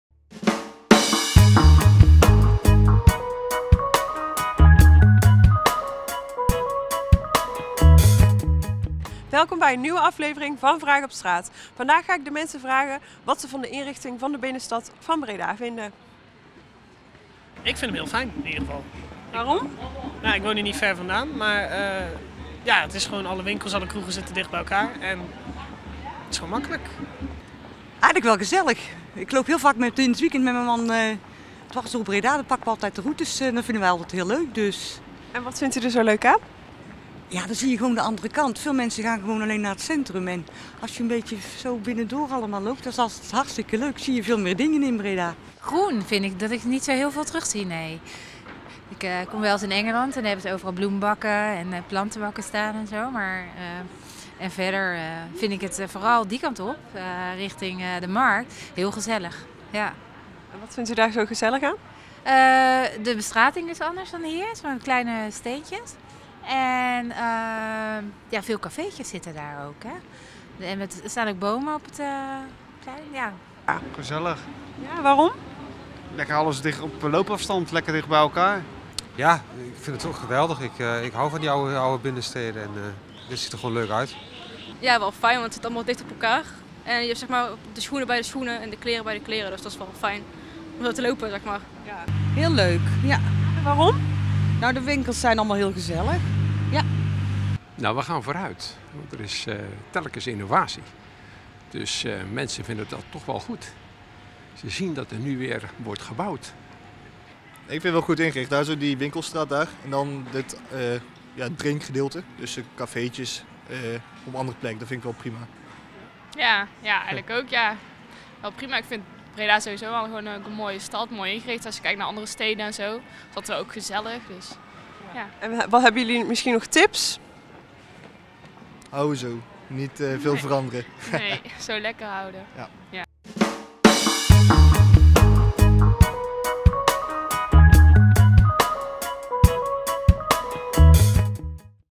Vraag op Straat: Inrichting binnenstad
We gingen de straat op met de vraag wat de mensen vonden van de inrichting van de Bredase binnenstad.